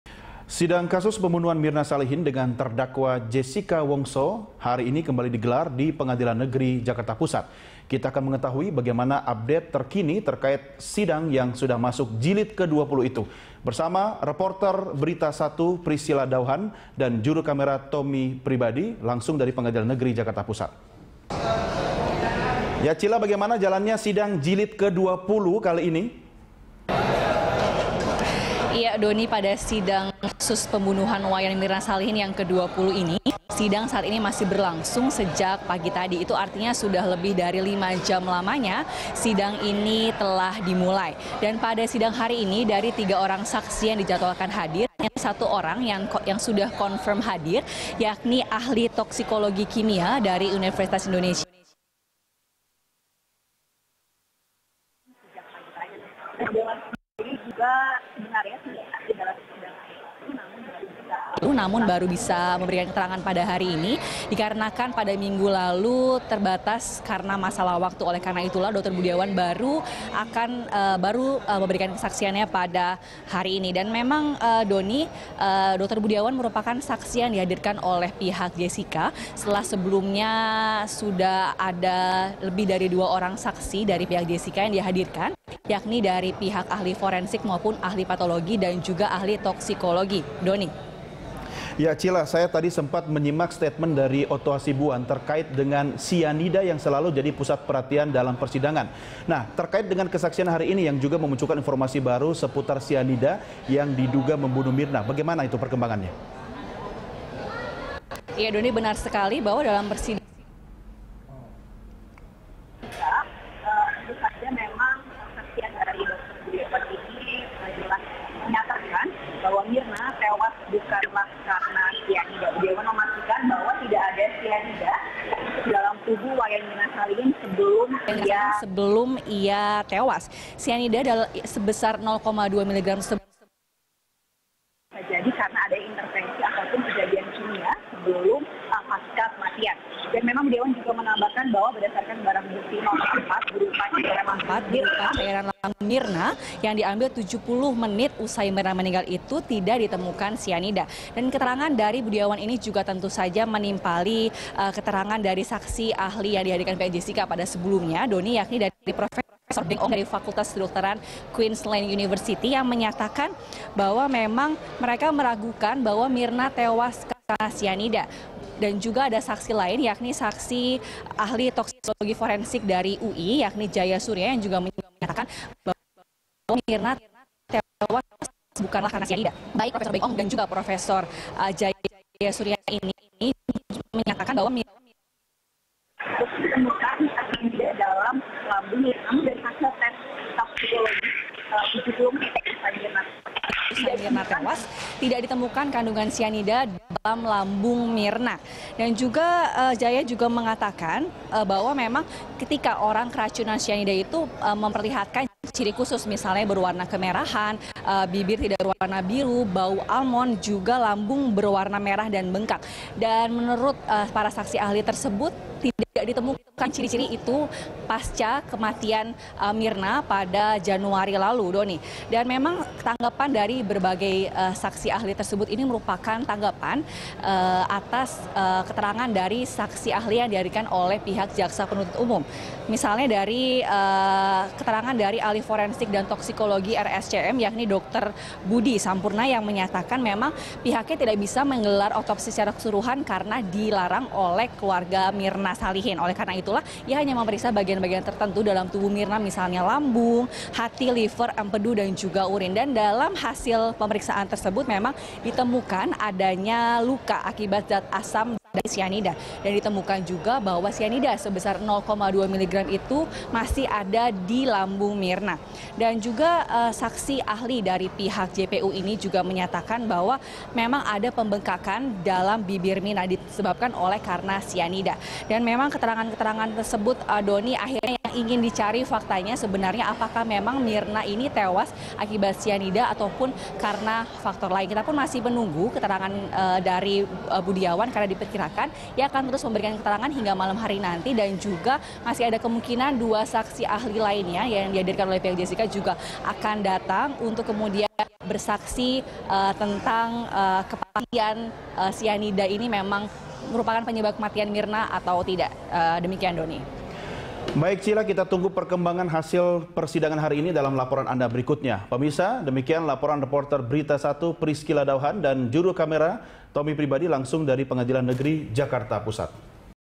Saksi Ahli Ragukan Sianida Penyebab Kematian Mirna
Sidang kasus pembunuhan Mirna Salihin dengan terdakwa Jessica Wongso kembali digelar di Pengadilan Negeri Jakarta Pusat. Sidang sudah masuk jilid ke-20.

Facebook.com/BeritaSatuTV
Youtube.com/BeritaSatu